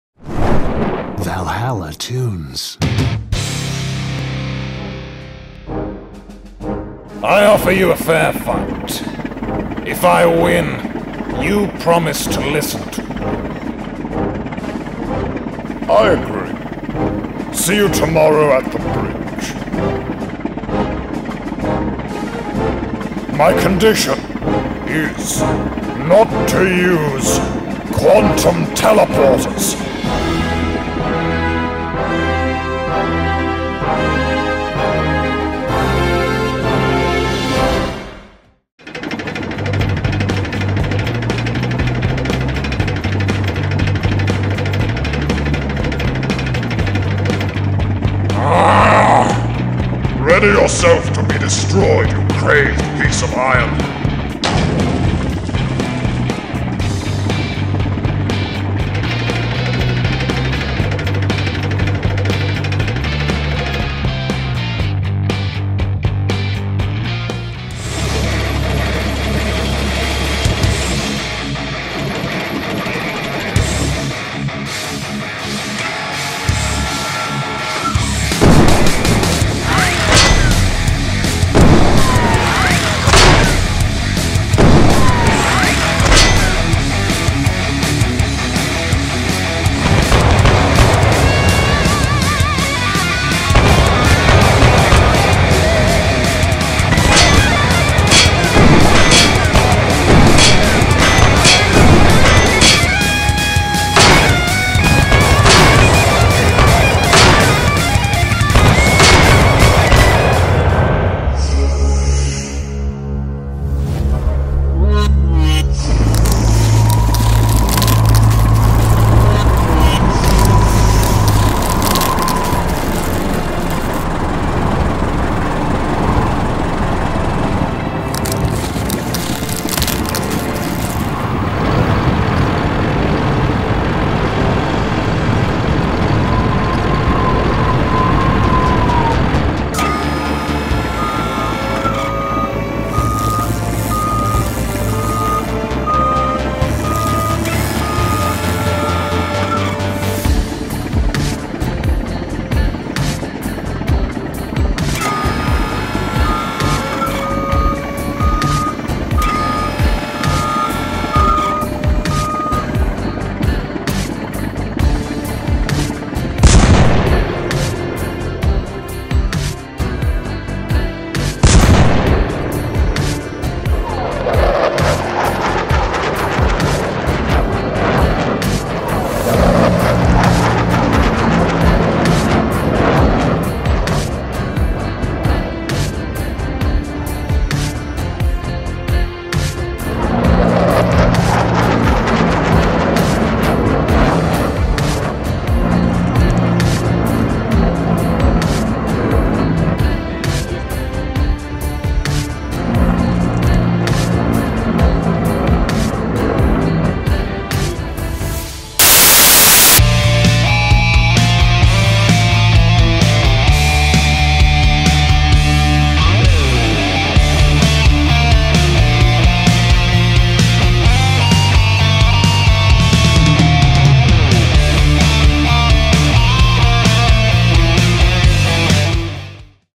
"The King vs Black Dorian" Cartoons about tanks
In this episode, you will see the beginning of a battle between two super-powerful tanks. King Dorian will meet Black Dorian. These two iron monsters have almost identical technical characteristics. Therefore, the outcome of the battle is very difficult to predict. Moreover, this is only the beginning of their tank duel. But King Dorian, like his rival, has special reserves. In general, if you like cartoons about tanks, then I hope this video will interest you. valhalla_toons tanks # cartoons_about_tanks world_of_tanks_animation # tanks_animation